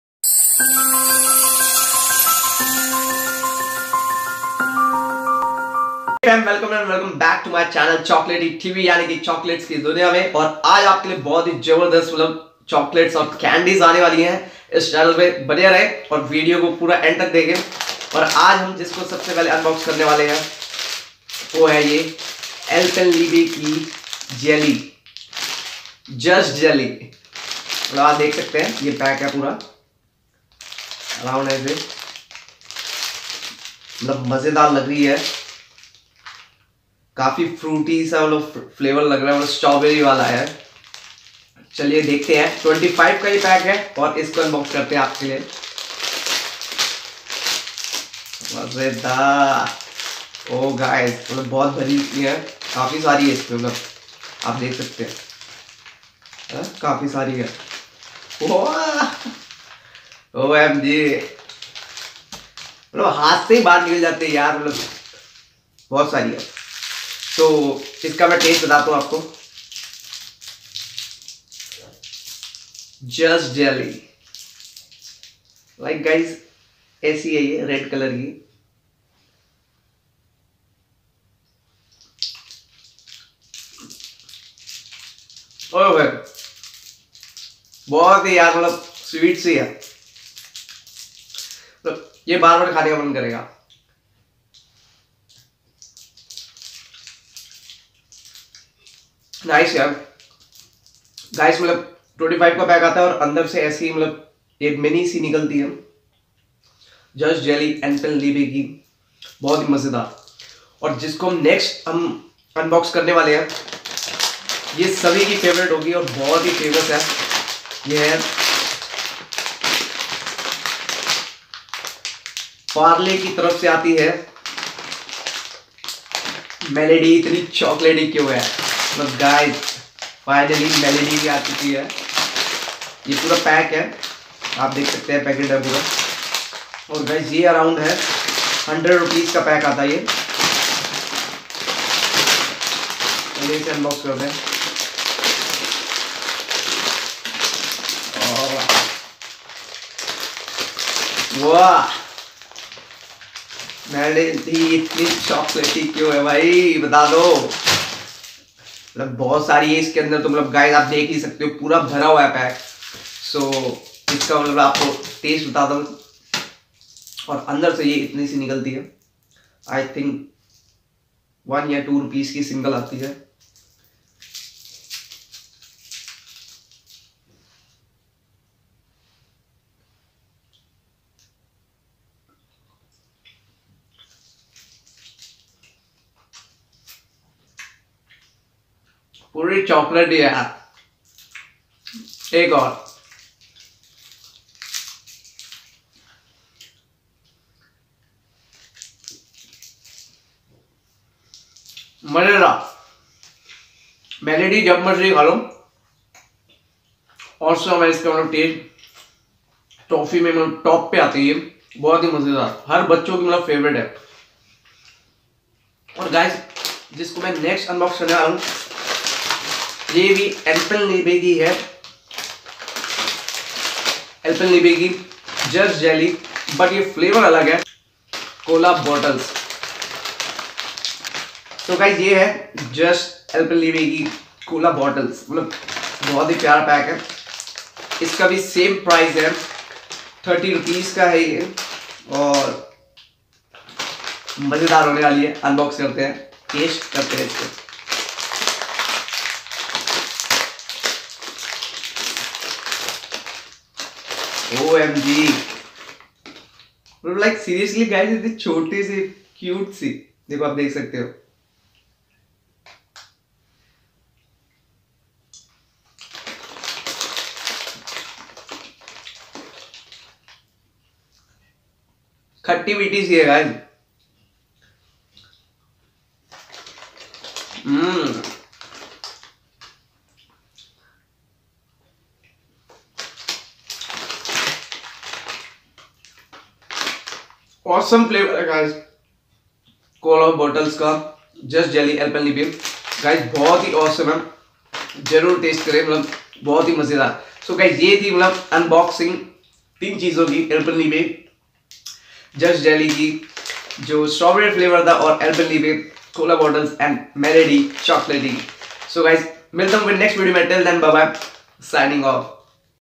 Alpenliebe Just Jelly, Parle Melody Chocolaty, Just Jelly Cola Bottle Unboxing
CHOCOLATES:
1. Alpenliebe Just Jelly Strawberry
2. Parle Melody Chocolaty
3. Alpenliebe Just Jelly Cola Bottle

Intro.                                      00:01
Alpenliebe Just Jelly.   00:29
Melody Chololaty.          02:00
Just Jelly Cola.                03:50